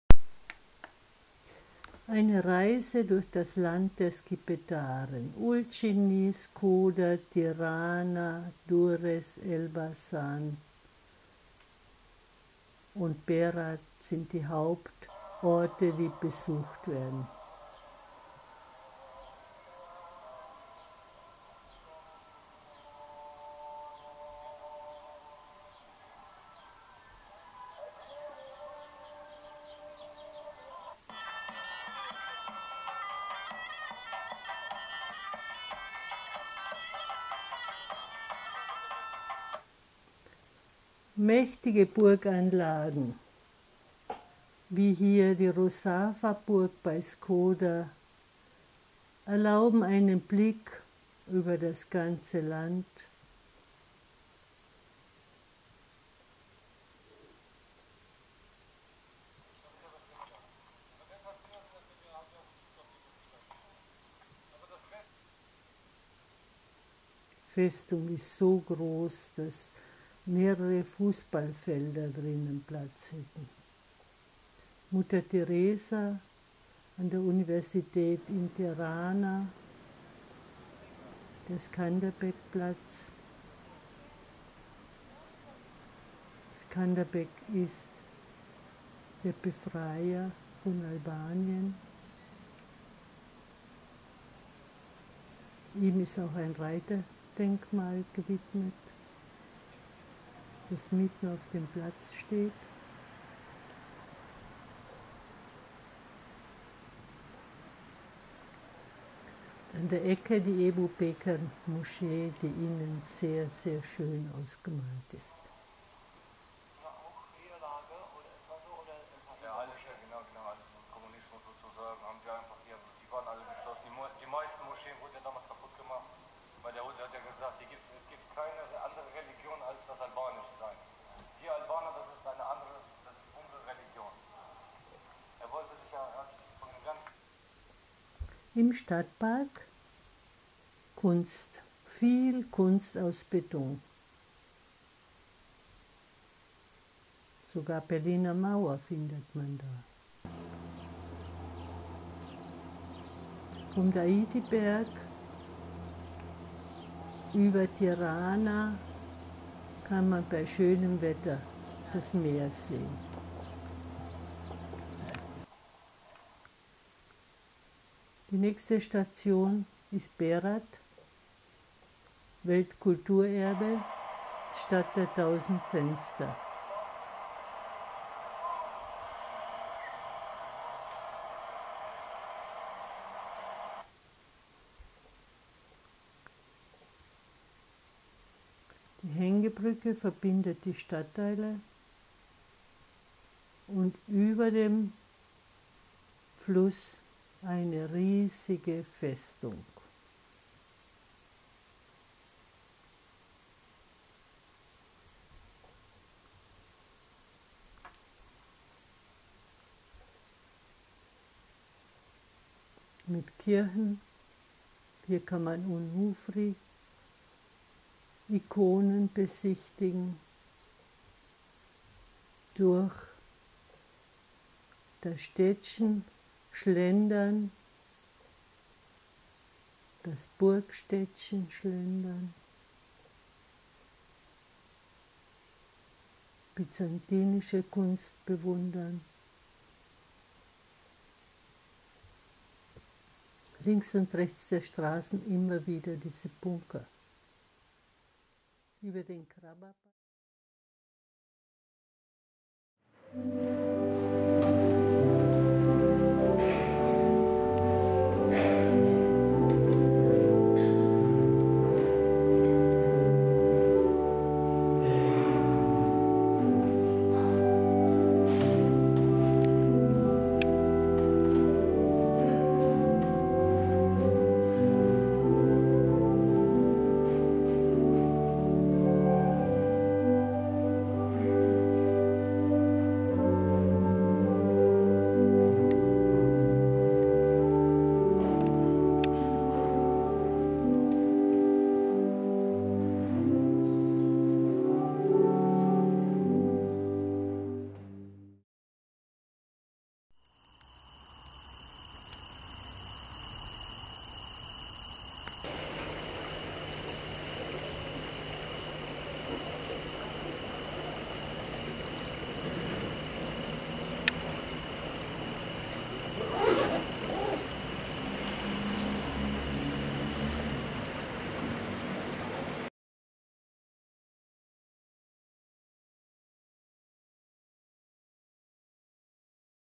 2015 Skipetaren
Albanien: Ein Besuch im Land der Skipetaren. Shkodra, Tirana, Berat, Elbasan im Juni 2014.